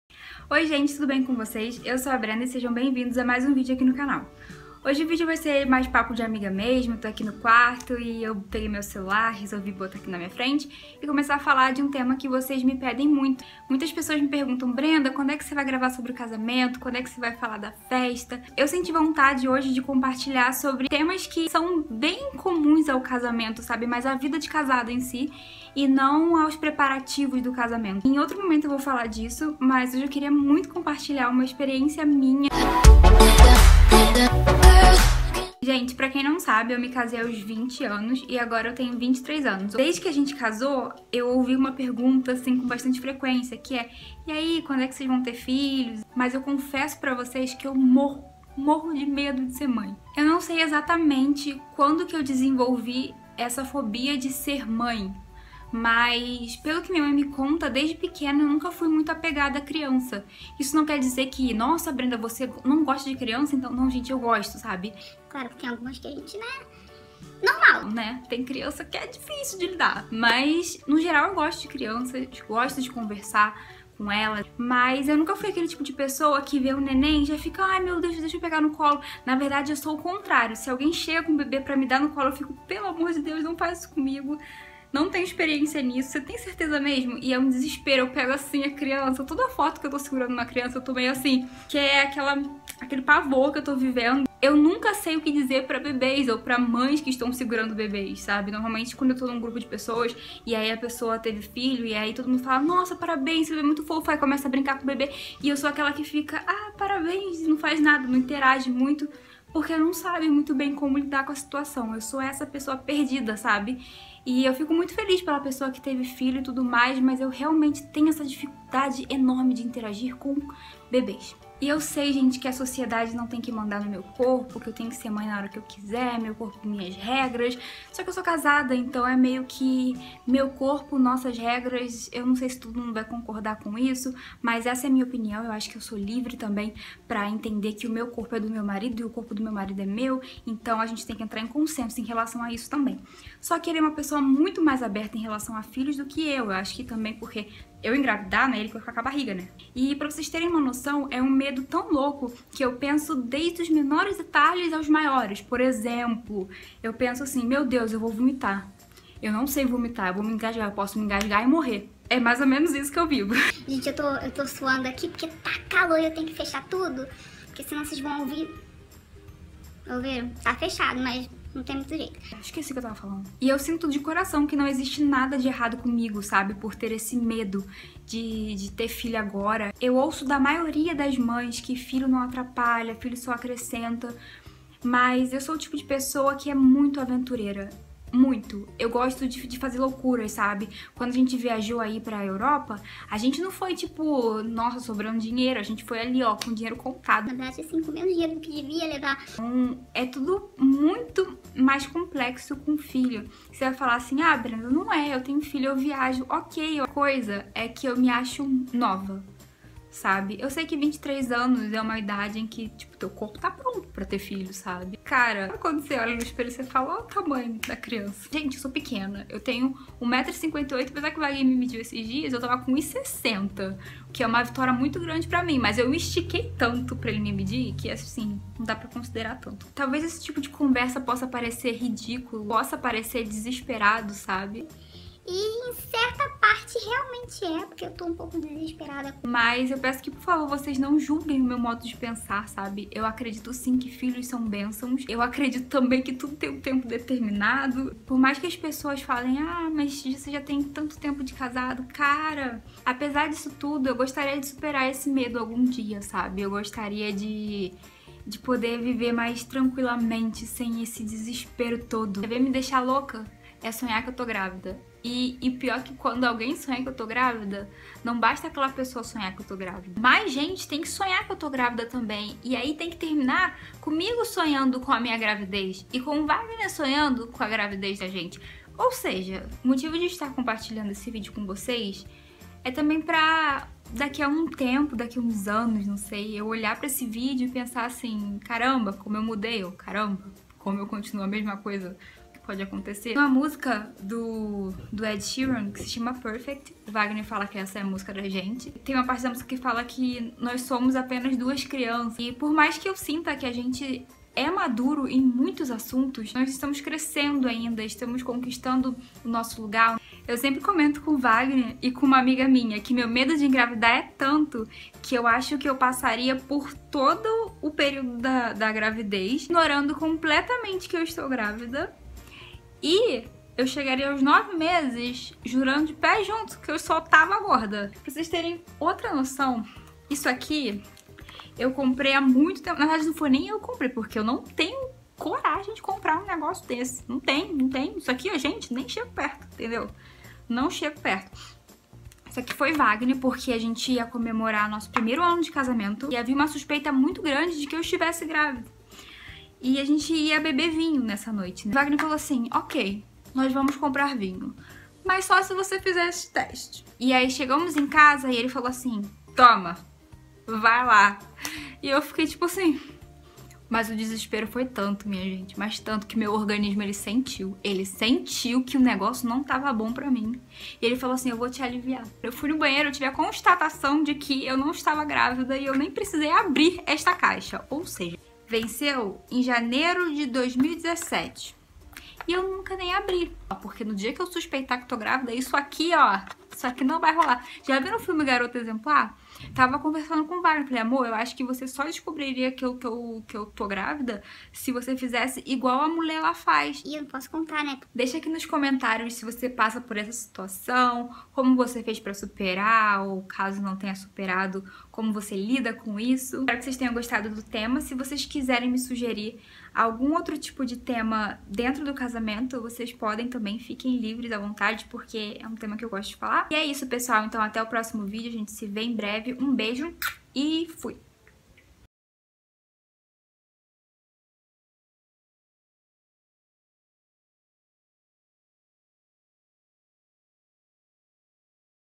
CASEI AOS 20 | Medo de ENGRAVIDAR
Hoje, a gente começa uma nova série por aqui. O "Casei aos 20" vai contar minhas experiências e confissões como esposa. Eu espero que vocês gostem!